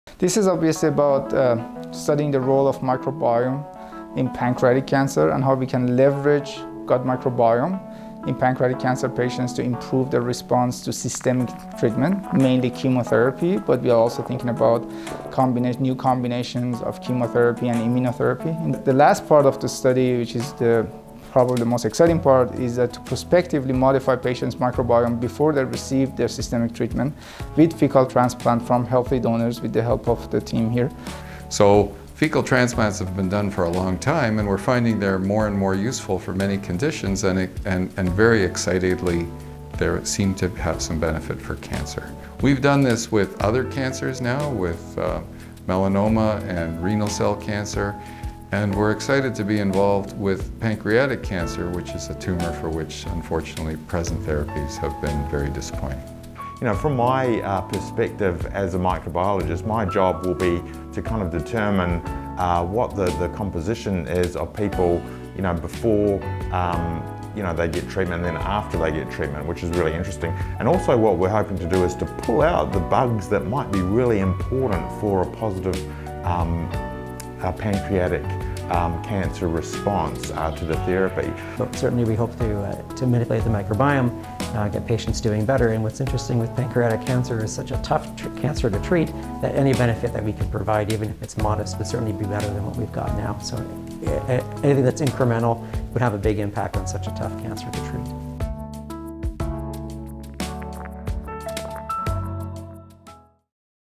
How does gut bacteria play a roll in pancreatic cancer?
Pancreatic cancer is an aggressive form of cancer that affects up to six-thousand Canadians a year and is the third leading cause of cancer deaths.
Due to its aggressive nature, pancreatic cancer is very hard to treat, partially because it is often detected at a later stage, and has the tendency to spread very quickly within the body.